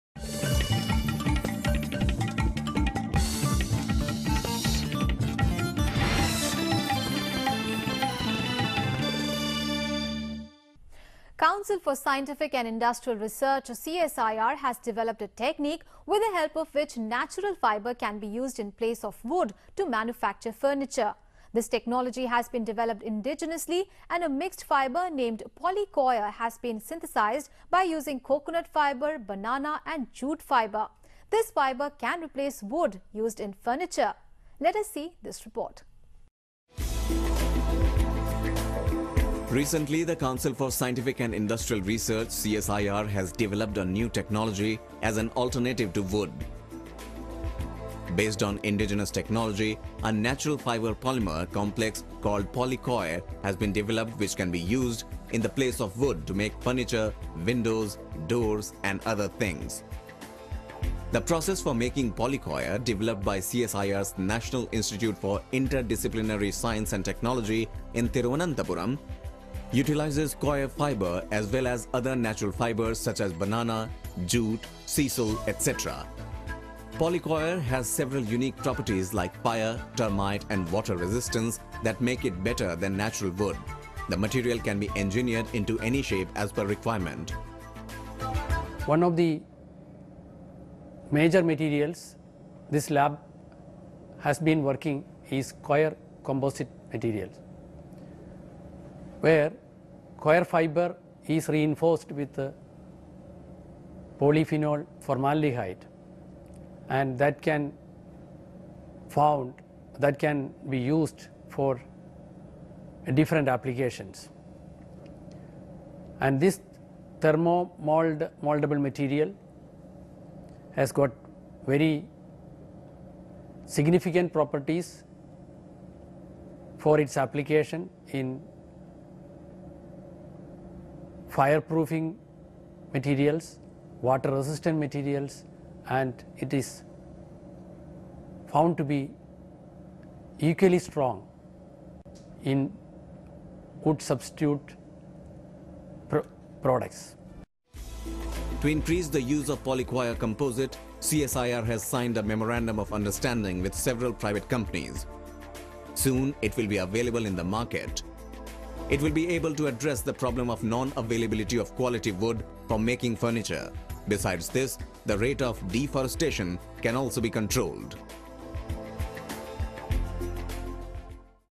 Polycoir
This video describes a technique developed by CSIR by which we can protect our forests. They described how natural fibre can be used as an alternative to woods. Scientists have developed polycoir from banana, jute etc. to manufacture furniture